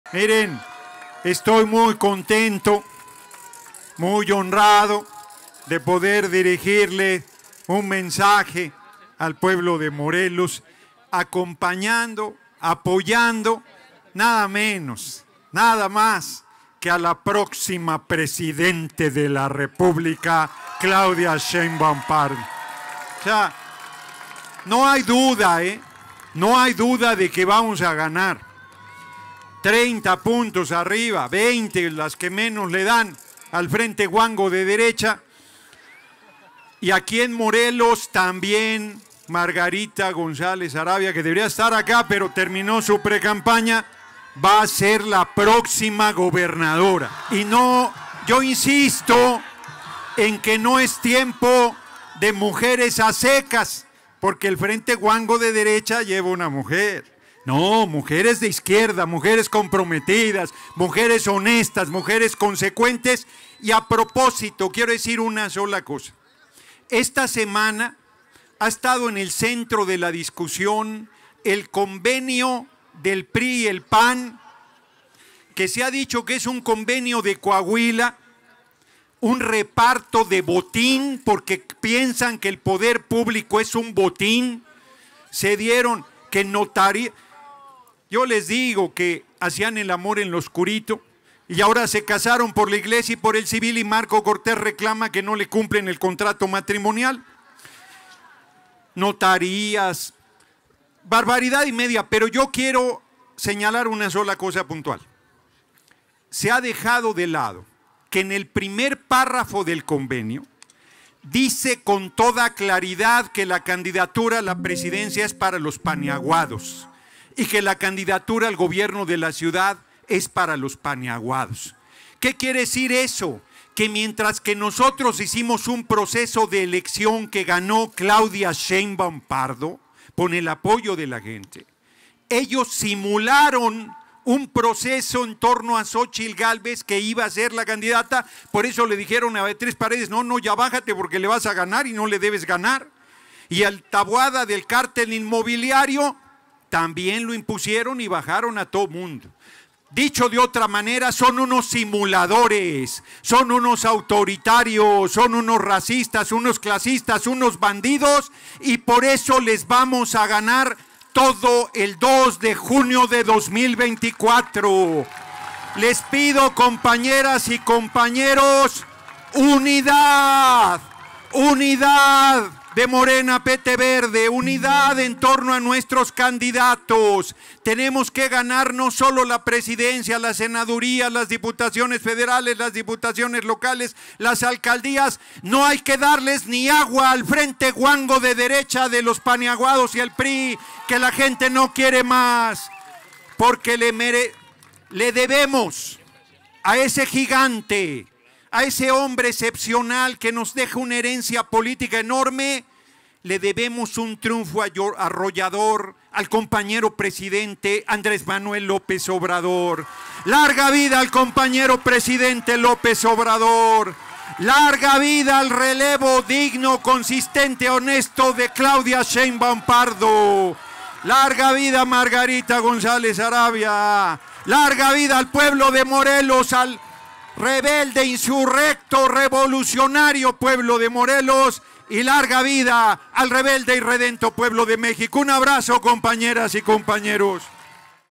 Noroña acusa simulación del PRI y PAN en las elecciones
En Cuernavaca, Morelos, Gerardo Fernández Noroña detalló que en el convenio compartido por Marko Cortés, presidente del PAN, “ellos simularon un proceso en torno a Xóchitl Gálvez  que iba a ser la candidata, por eso le dijeron a Beatriz Paredes no, no, ya bájate porque le vas a ganar y no le debes ganar”